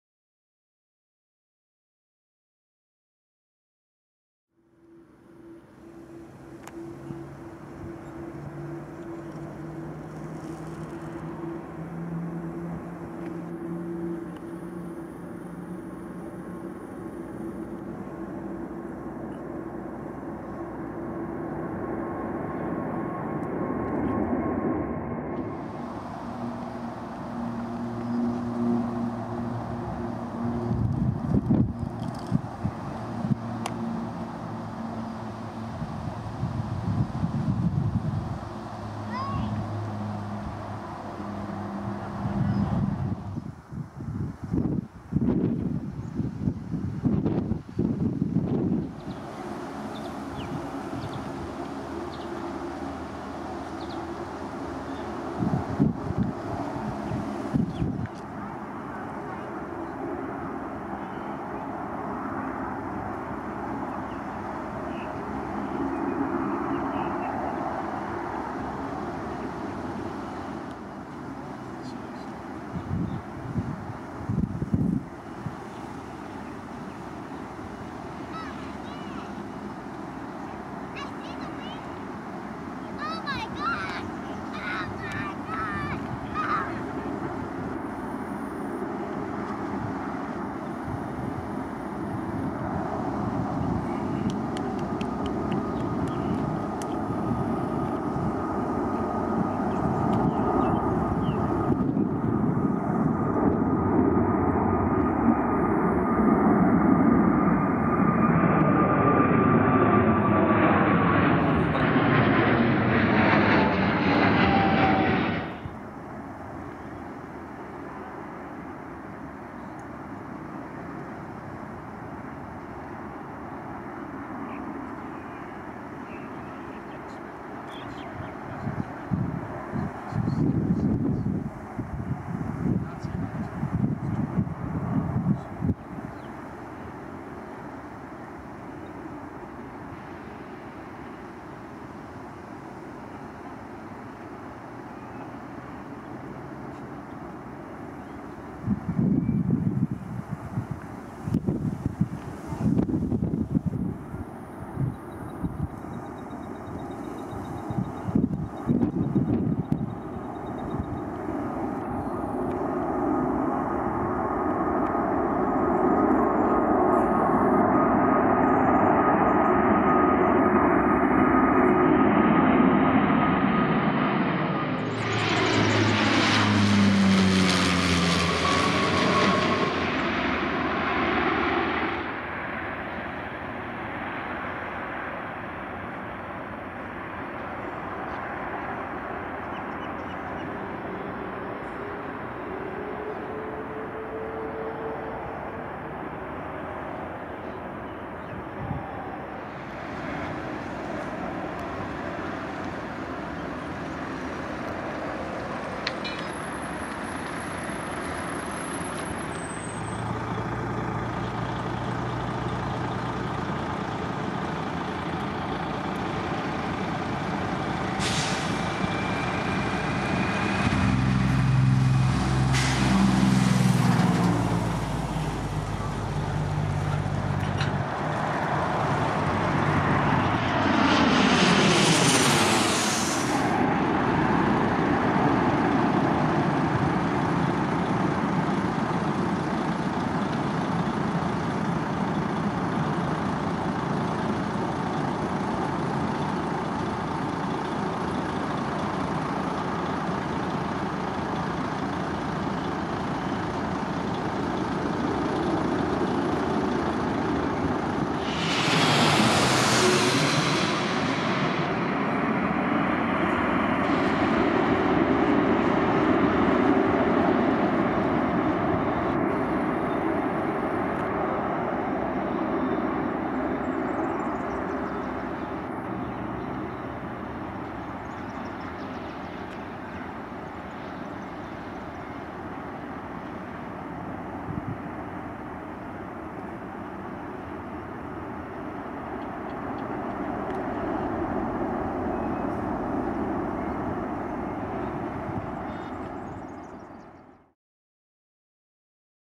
COMPILATIONS TAKE OFF AND LANDING
Afternoon plane spotting at Sydney airport
Runways 16R/34L.

This video is the property of G U R I T N O  and may not be reused without permission.